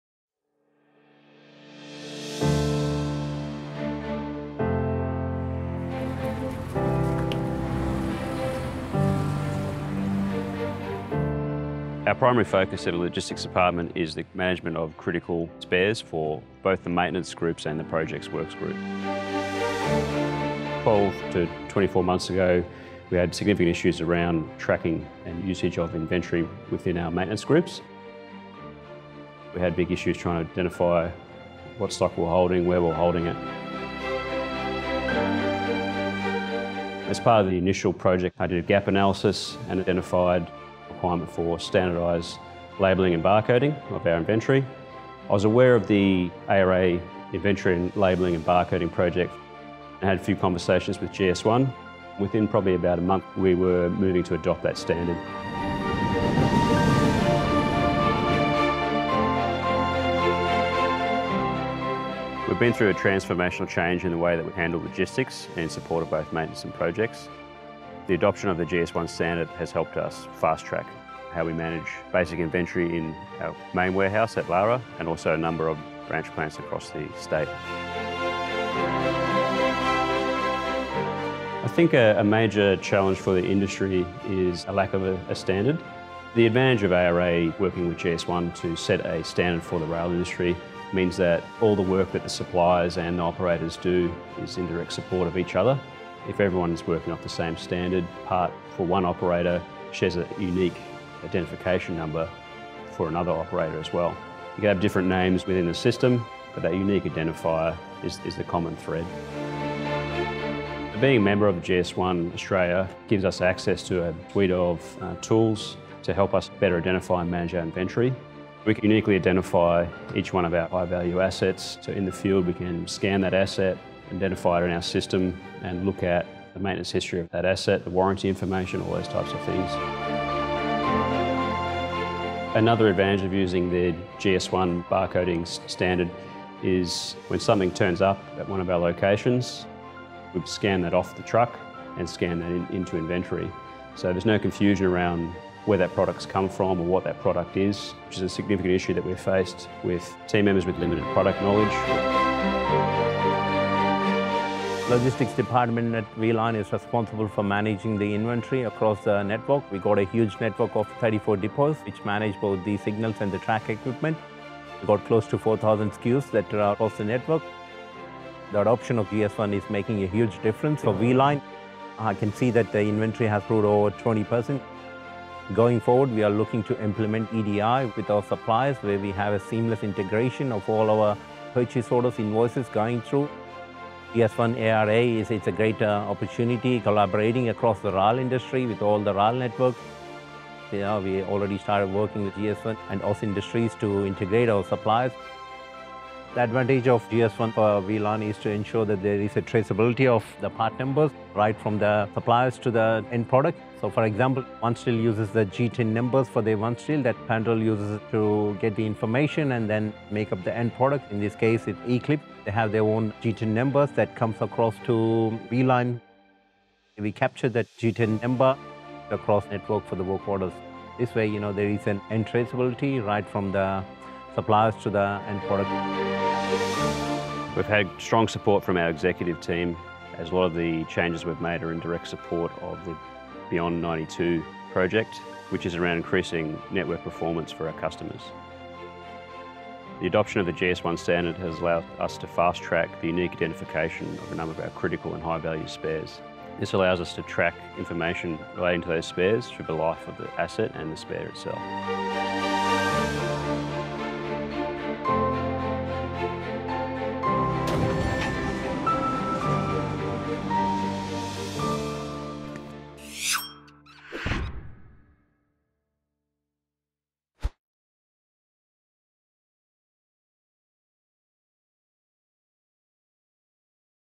V/Line – A Project i-TRACE success story
Project i-TRACE – setting the foundation for effective Life Cycle Management (LCM) of parts, components and assets in the rail sector.

V/Line have embraced Project i-TRACE and are leading the industry with the implementation of the GS1 standards. V/Line are already seeing the benefits of working with the ARA and GS1 Australia on the standardised identification and marking of parts and components.